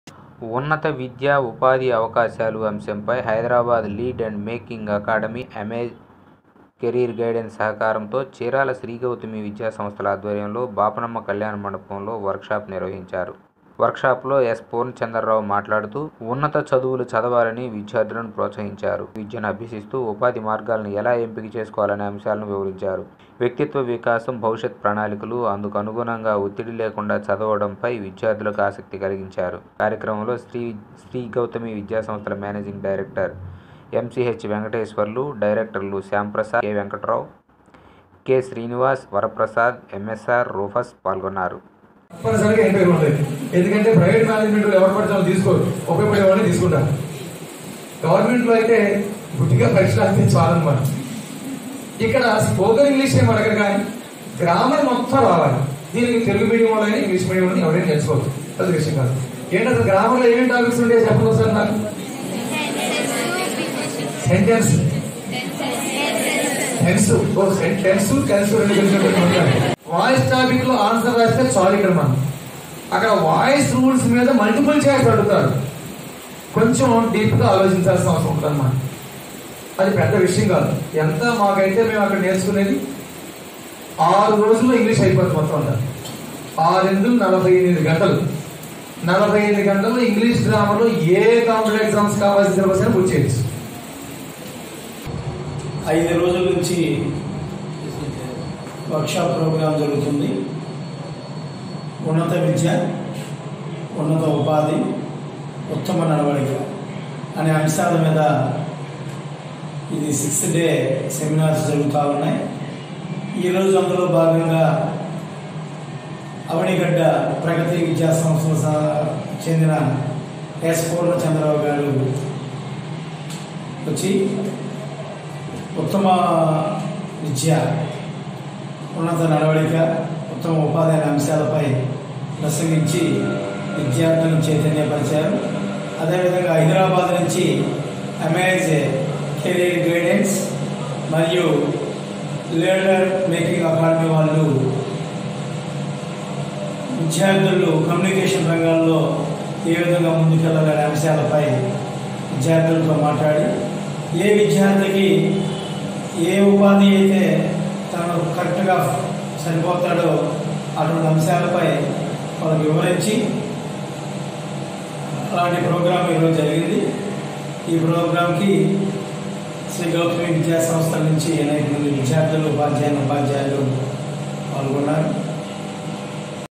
Chirala Sri Gouthami Organised workshop on higher education || DN5 News
ఉన్నత విద్య ఉపాధి అవకాశాలు అంశంపై హైదరాబాద్ లీడర్ అండ్ మేకింగ్ అకాడమీ, అమేజ్ కెరీర్ గైడెన్స్ సహకారంతో శ్రీ గౌతమి విద్యాసంస్థల ఆధ్వర్యంలో చీరాల బాపనమ్మ కళ్యాణ మండపంలో వర్క్ షాప్ నిర్వహించారు. వర్క్ షాప్ లో ఎస్ పూర్ణచంద్రరావు మాట్లాడుతూ ఉన్నత చదువులు చదవాలని విద్యార్థులను ప్రోత్సహించారు. ఉన్నత విద్యను అభ్యసిస్తూ ఉపాధి మార్గాలను ఎలా ఎంపిక చేసుకోవాలనే అంశాలను వివరించారు. వ్యక్తిత్వ వికాసం, భవిష్యత్ ప్రణాళికలు, అందుకు అనుగుణంగా వత్తిడిలేకుండా చదవడంపై విద్యార్థులకు ఆసక్తి కలిగించారు. కార్యక్రమంలో శ్రీ గౌతమి విద్యాసంస్థల మేనేజింగ్ డైరెక్టర్ ఎంసీహెచ్ వెంకటేశ్వర్లు (ఎంవి), డైరెక్టర్ లు శ్యామ్ ప్రసాద్, కె వెంకటేశ్వర్లు(కెవి), కె శ్రీనివాస్(ఏఓ), వరప్రసాద్, ఎమ్యెసార్, రూఫస్ పాల్గొన్నారు.